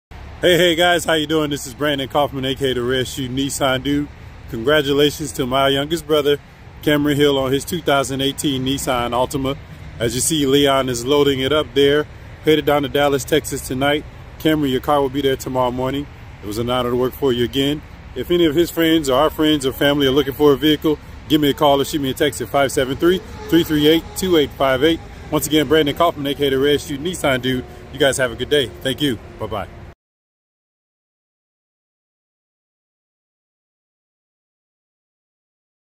Congrats to Mr. Hill!!
Congrats to Mr. Kameron Hill on his 2018 Nissan Altima!! This one will be arriving in Dallas, TX tomorrow morning.